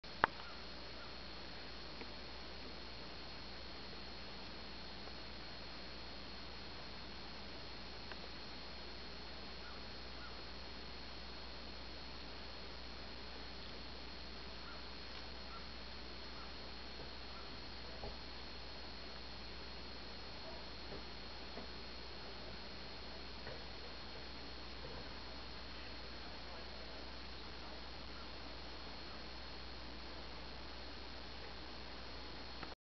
Fruit Loving Cat
Yukon likes bananas